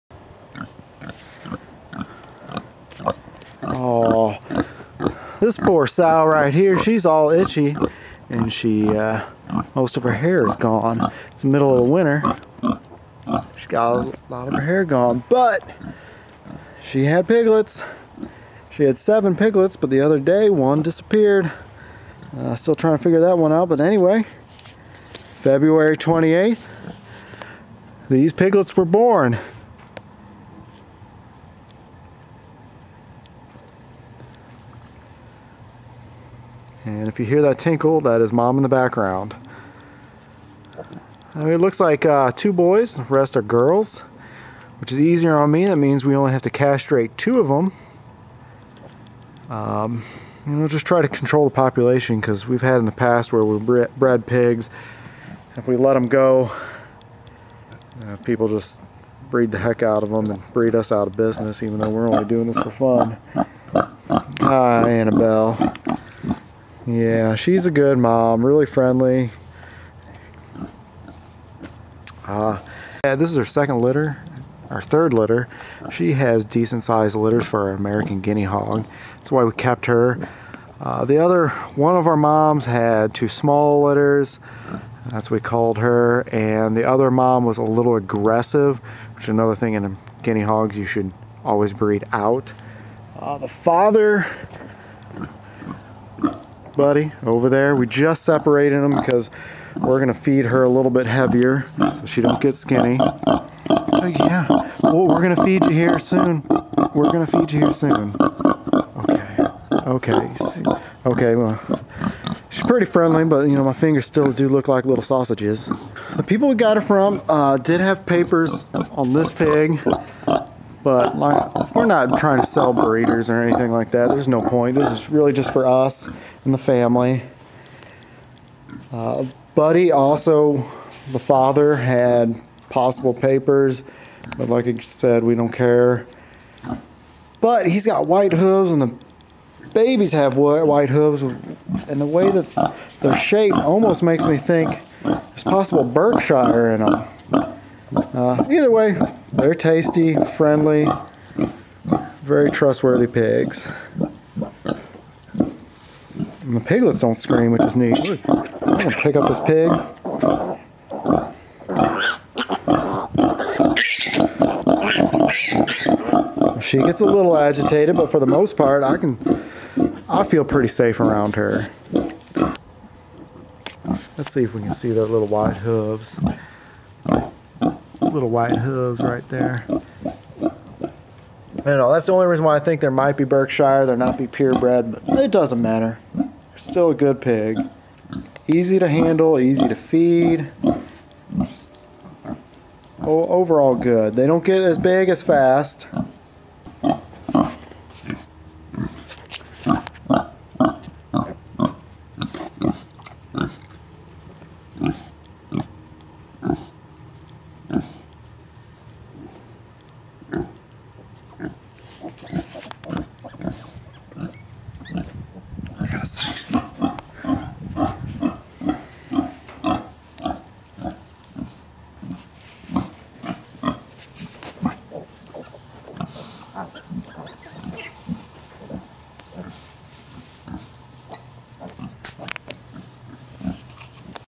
American Guinea Hog piglets are here...again!
This is our 10th AHG litter. This time its about quality not quantity.